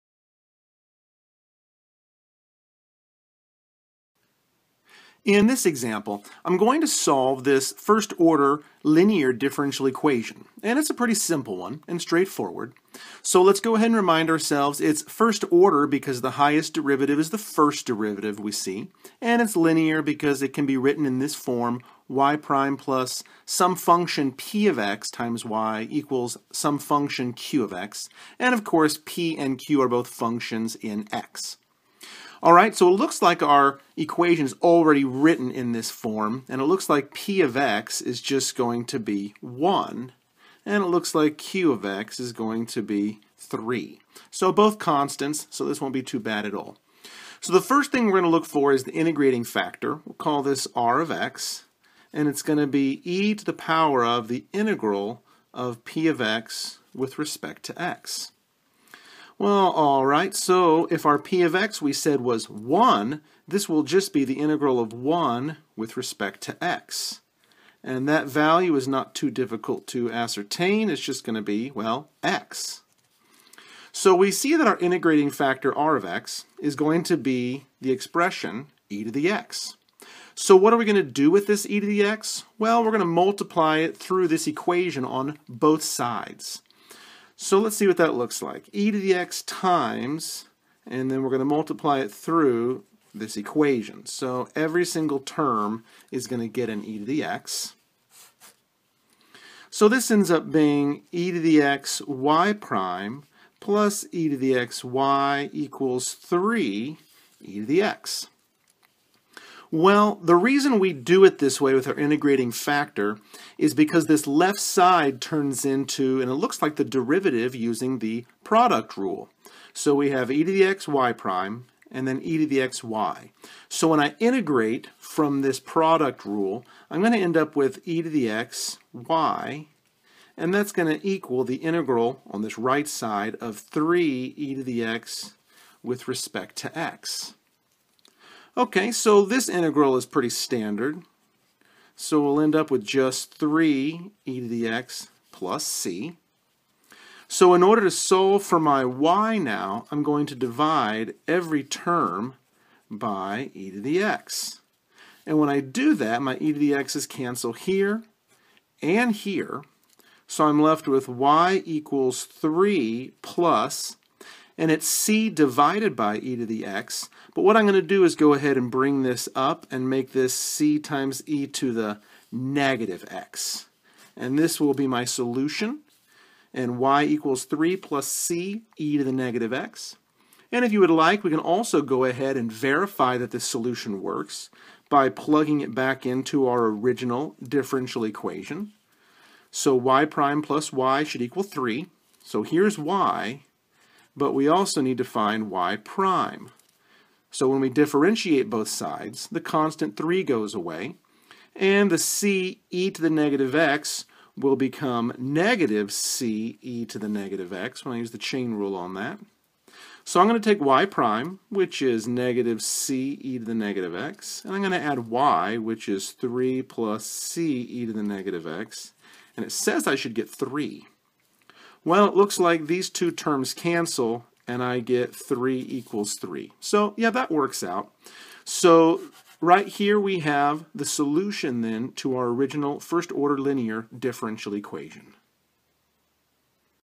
First Order Linear Differential Equation (Example)
This video walks through an example to find the general solution of a first-order linear differential equation using the integrating factor.